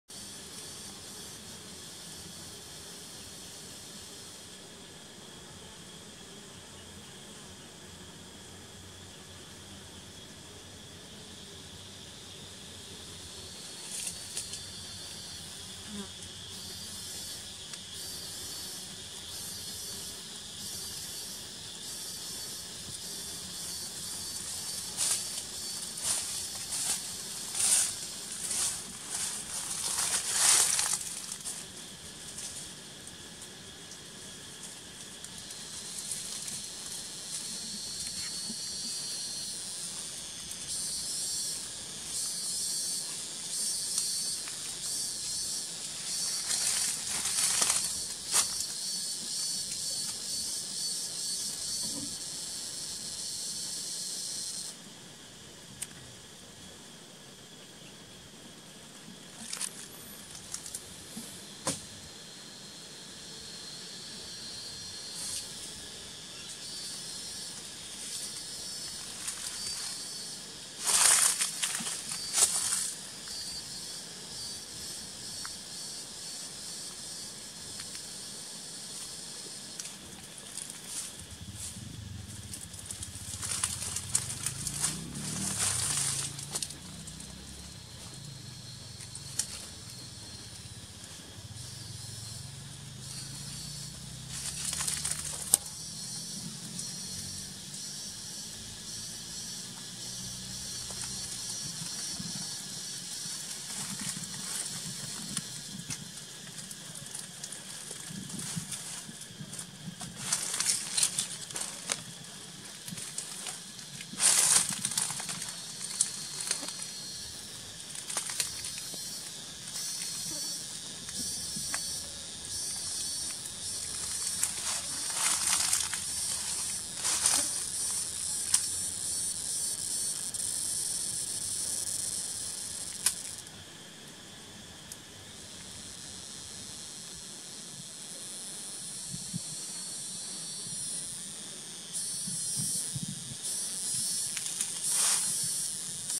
Khmer Beekeeping Apiary on April 08, 2019
Here is the view of my apiary on April 08, 2019. It is simple, quiet and natural. I like it very much! I love simple, quiet and natural life. I follow my passion, and how about you? Please tell me what does your passion like.